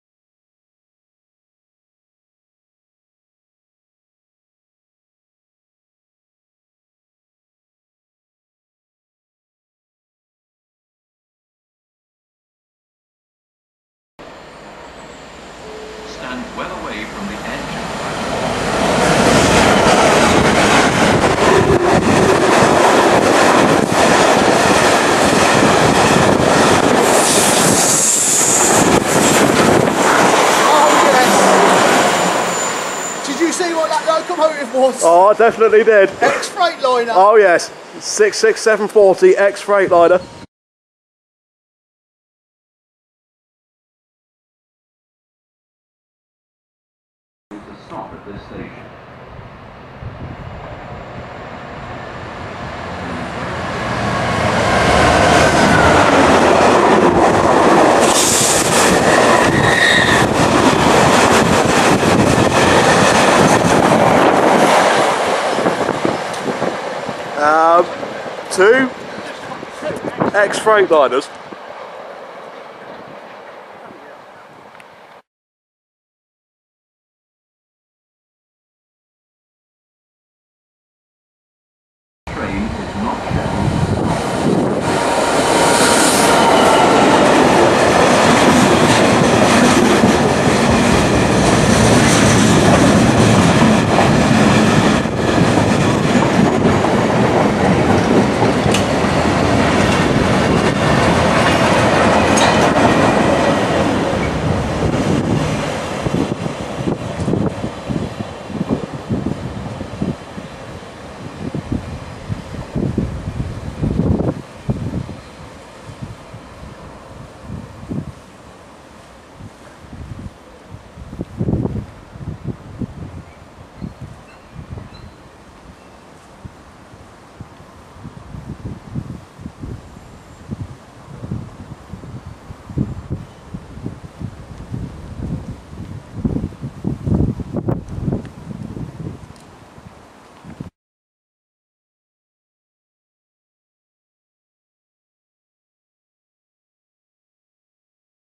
GBRf 'Sheds' at Godalming Station 11/12/2011
A small selection of GBRf engineers trains that passed through Godalming Station on a bitterly cold Sunday. Each train was running from a worksite near Fareham back to Eastleigh Yard.

First up is 66740 (which was formerly Freightliner 66580), seen working a slightly late 6G11. Although unbranded, this is the first time a green '66' has been seen on the Portsmouth Direct. However, this wasn't to be the only one.

The next train to appear was 66739, another former Freightliner 'Shed' (it used to be 66579). This was working 6G12 which was also running slightly behind time.

With the weather turning in, and with both myself and ChuffersProductions about to give up (it was that cold out), 66714 'Cromer Lifeboat' took us almost by surprise with a very late running 6G13. This was to be the final train we caught on film as we were both frozen by the time this one eventually turned up!

Thanks to ChuffersProductions for joining me, and also thanks to Jonnanna for the information on the engineers trains :)